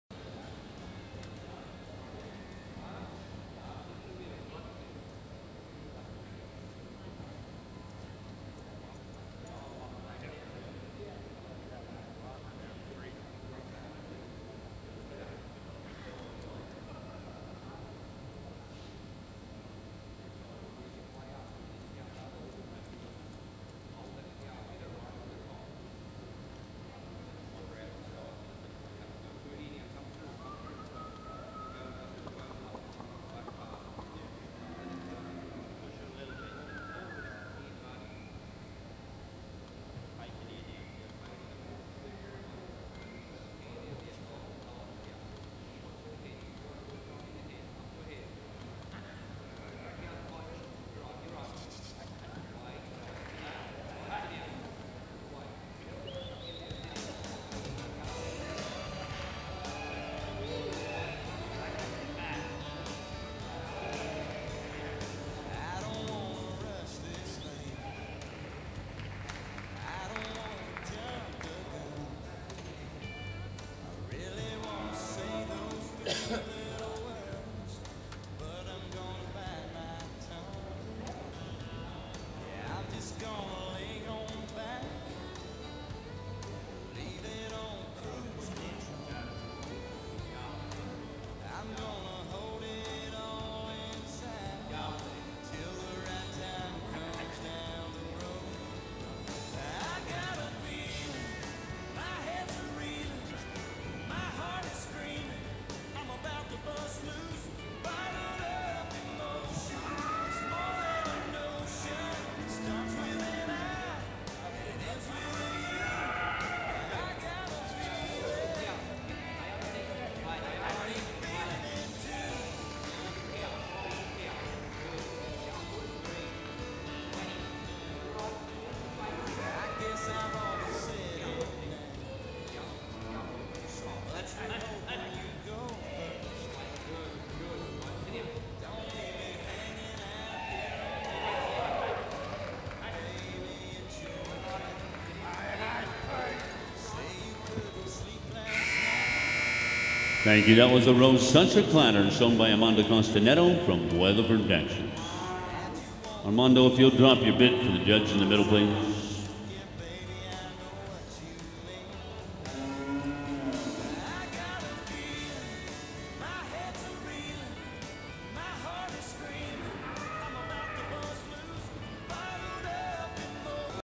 Arosesuchaclatter - 5-6yo Non Pro - 1st Go Round
NCHA Super Stakes 2012
Arosesuchaclatter (SMOOTH AS A CAT x MATES IRISH ROSE x SMART MATE)
Shown by Armando Costa Neto